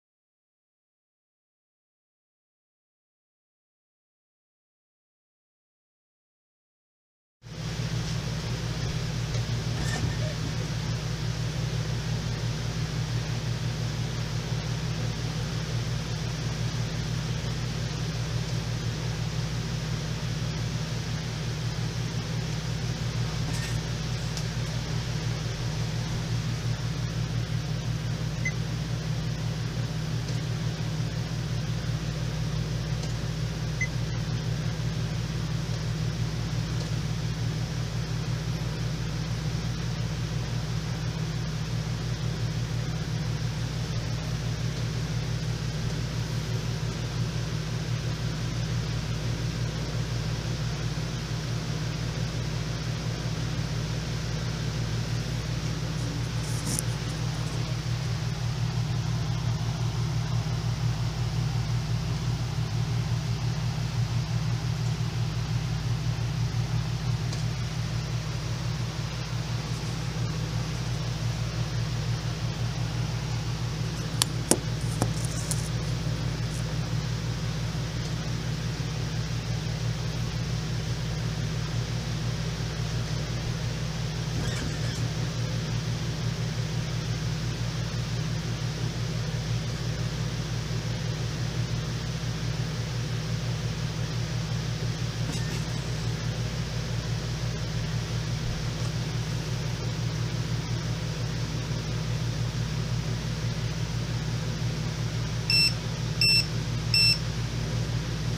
Hydrogen Rich Water 2
Low Cost Molecular Hydrogen Rich Water Maker purchased from TomTop has 3rd generation nano electrolysis technology. Mobile and works as advertised. After 3 successive generations, I was able to get the following: pH =  7.6 ,  H2 = 1.2 ppm, ORP = - 519 using pH drops, H2 Blue Reagent and a ORP Meter for values.